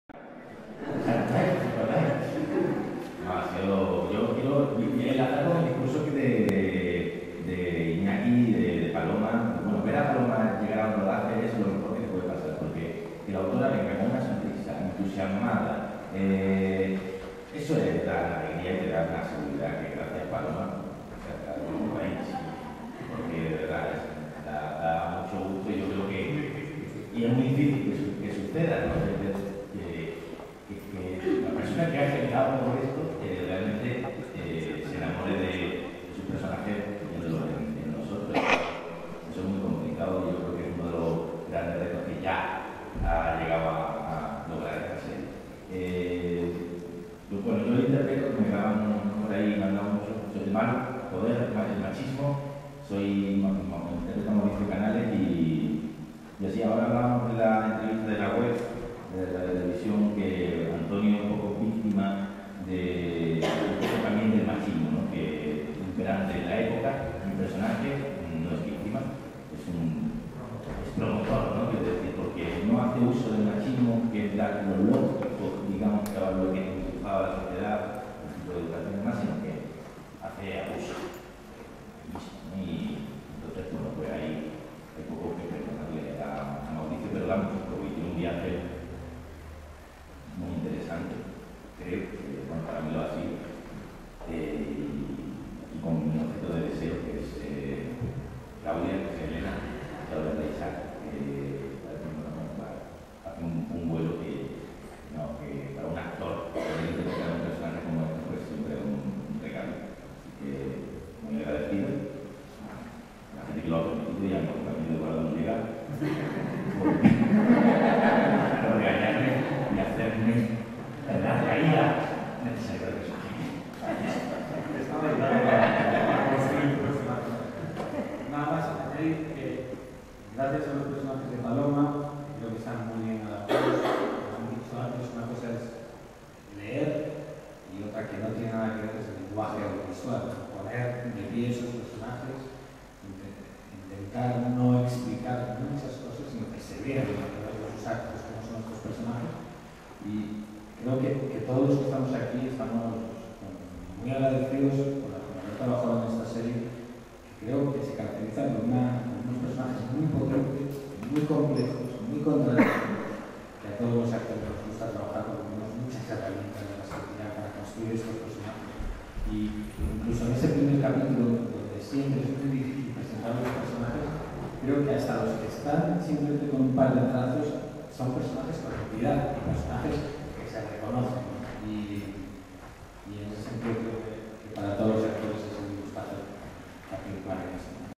FesTVal de Vitoria | Fran Perea encantado con La Sonata del Silencio
Fran Perea vuelve a la pequeña pantalla con un nuevo personaje de ficción. Se mostró encantado con La Sonata del Silencio, nuevo estreno de TVE. La presentación tuvo lugar dentro de la octava edición del FesTVal en Vitoria.